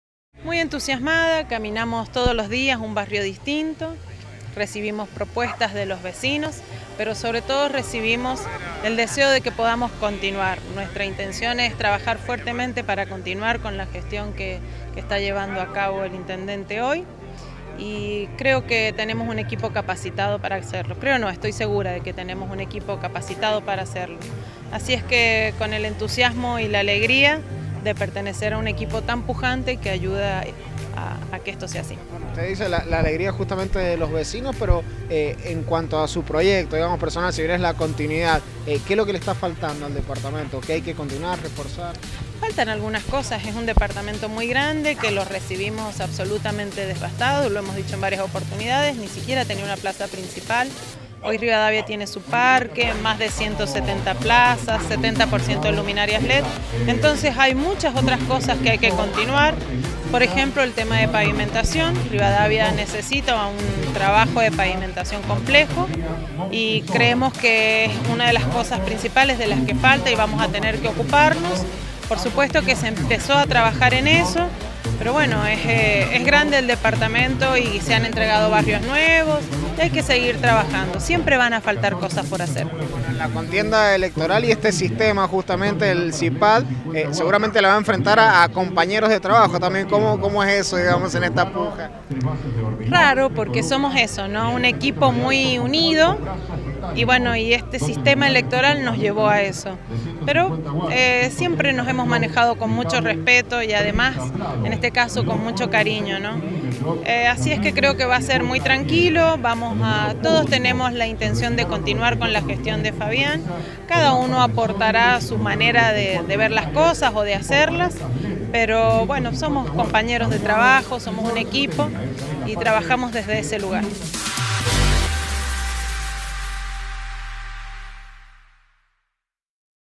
Candidatura a intendenta: 'la continuidad conmigo apuntará a la pavimentación
Nancy Picón, diputada provincial, candidata a intendenta por Rivadavia, dialogó con el móvil de Canal 13.